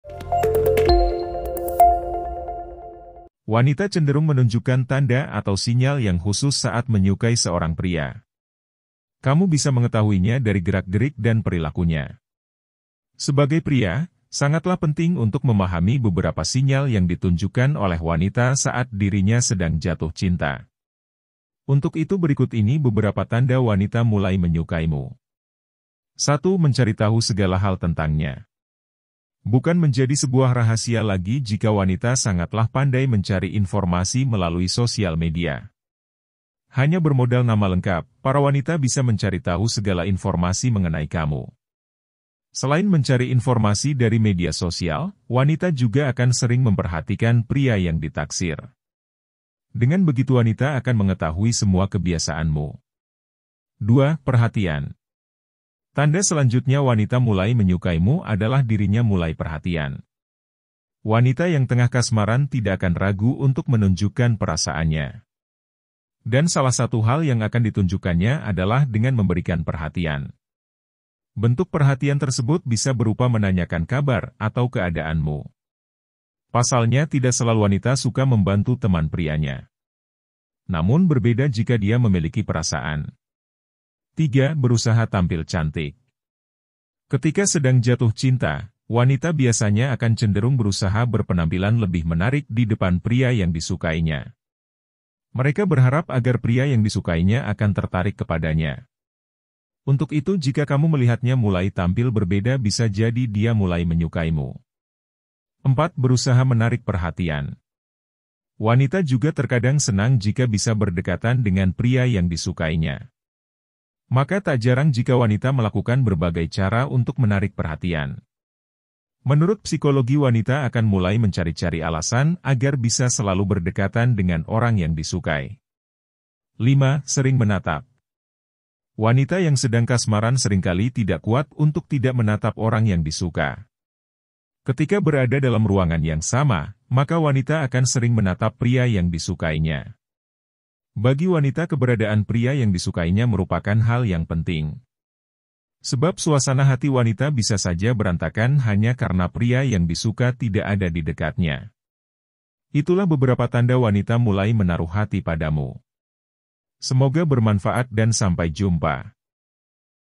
Tanda Jelas Wanita Mulai Menyukaimu
Wanita cenderung menunjukkan tanda atau sinyal yang khusus saat menyukai seorang pria. Kamu bisa mengetahuinya dari gerak gerik dan perilakunya. Sebagai pria, sangatlah penting untuk memahami beberapa sinyal yang ditunjukkan oleh wanita saat dirinya sedang jatuh cinta. Untuk itu berikut ini beberapa tanda wanita mulai menyukaimu.